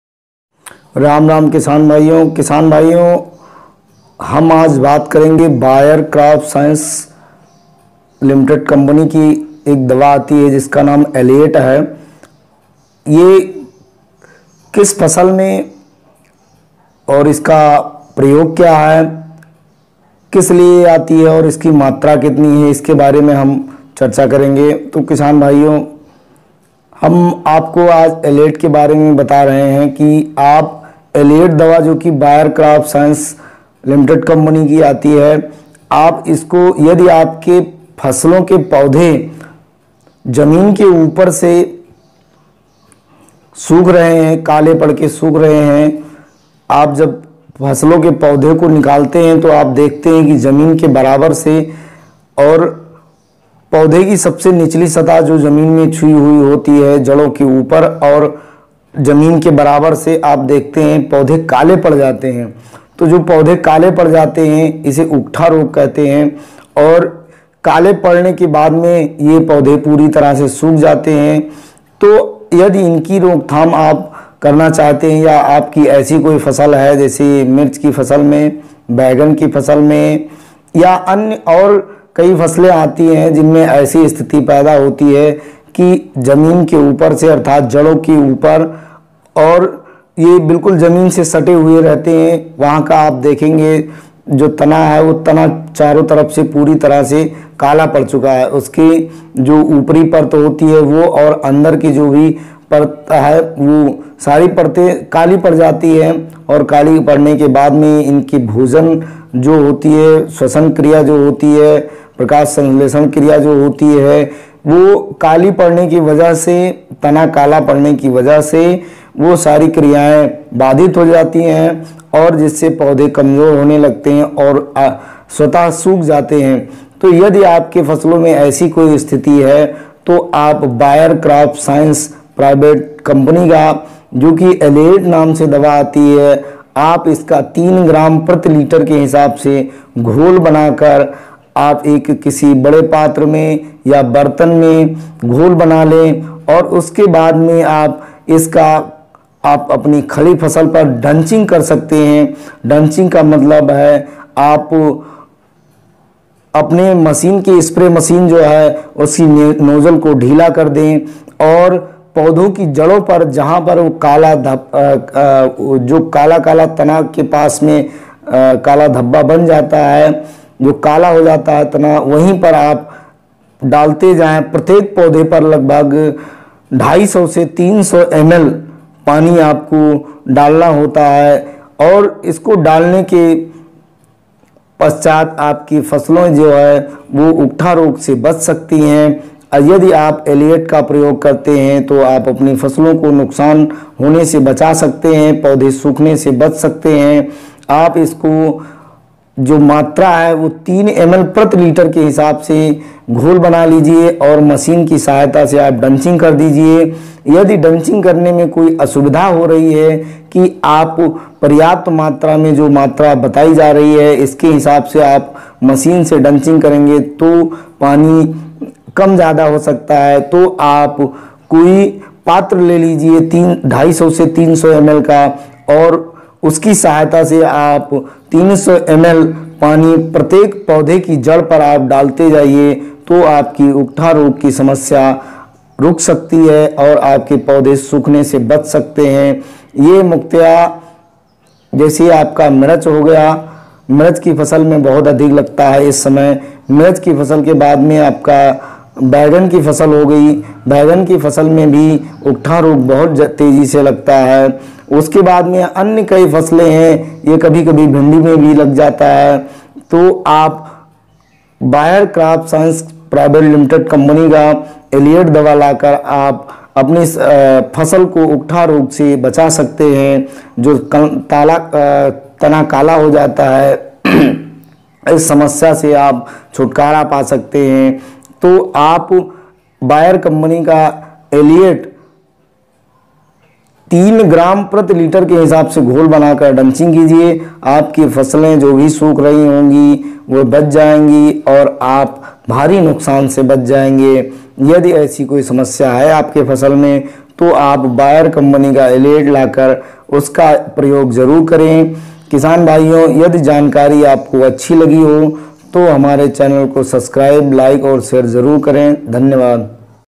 पौधों_मे_उकठा_रोग_की_रोकथाम_कैसे_करें Aliette_fungicide_का_प्रयोग Dr_Narendra_kumar_Yadav
उकठा रोग में आप Aliette दवा का प्रयोग करके आप इस  समस्या से अपनी फसलों को बचा सकते हैं
Dr Narendra kumar Yadav